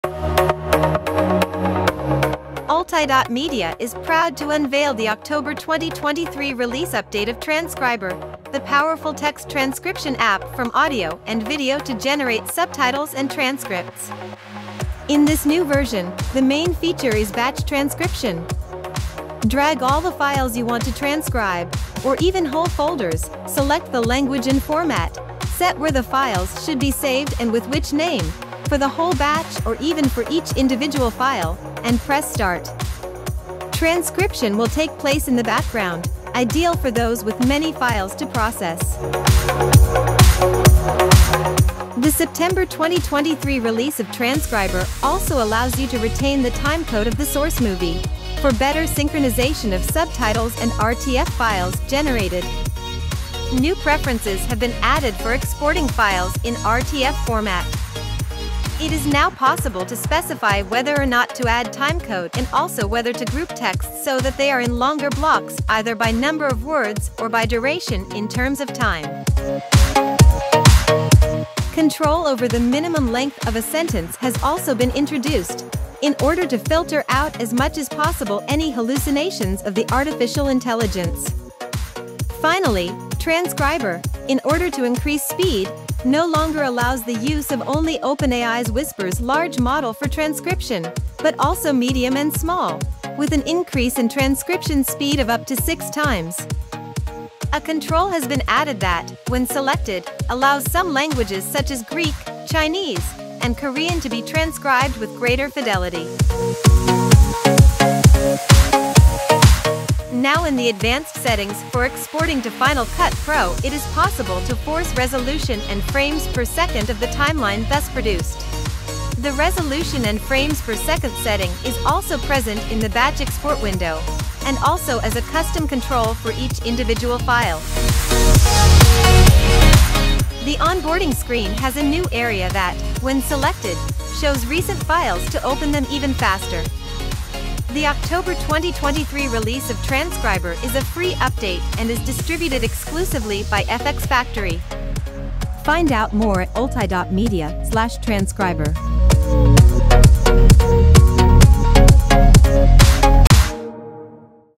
Transcriber Update October 2023 - Automatic Transcription and Subtitles Generator
Here is what's new in this awesome update:

1. **Batch Transcription:** Transcriber now supports batch transcription, allowing users to transcribe multiple files or entire folders at once. Users can select the language and format, specify where the files should be saved, and customize file names. Transcription occurs in the background, making it convenient for processing large numbers of files.

2. **Timecode Retention:** The new version retains the timecode of the source movie, aiding in better synchronization of subtitles and RTF files generated.

3. **New RTF Controls:** Enhanced preferences for exporting files in RTF format have been added. Users can choose whether to include timecode and group texts into longer blocks based on word count or duration.

4. **Artificial Intelligence Improvements:** Users can now control the minimum length of a sentence to filter out potential errors from the artificial intelligence. Additionally, Transcriber supports multiple models (Large, Medium, and Small) from OpenAI's Whisper, significantly increasing transcription speed.

5. **Language Fidelity:** A new control allows certain languages such as Greek, Chinese, and Korean to be transcribed with higher accuracy.

6. **FPS and Frame Size Control:** Advanced settings for exporting to Final Cut Pro now include options to specify the resolution and frames per second of the timeline. These settings are also available in the batch export window and for individual files.

7. **Recent Files:** The onboarding screen features a new section displaying recent files, enabling users to open them quickly for editing.